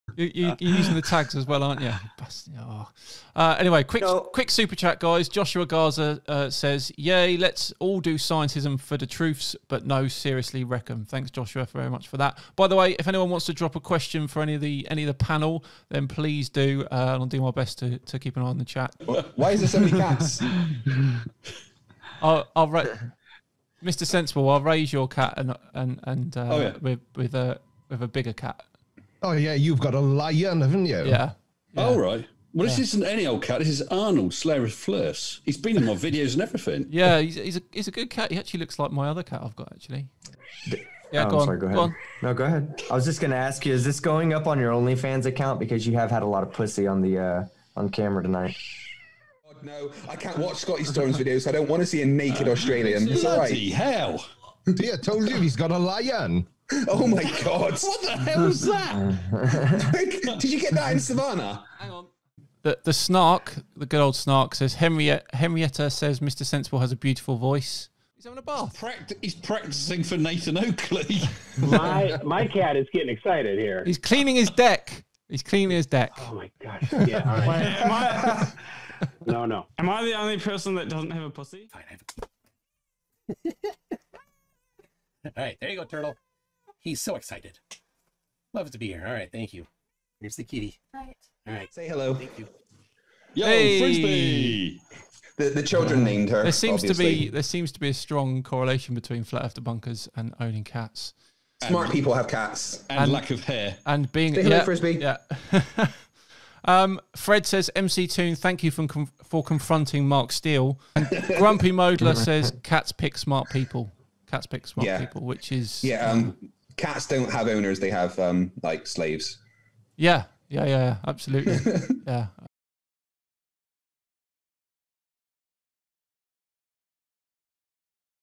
Debunkers have cats, new cat added
The original video dates back to the time when Team Sceptic did team-ups with Conspiracy Cats and SciManDan. The days of The Agent.
I left Team Sceptic in there, but added a cat that wasn't there in the original.
Have fun!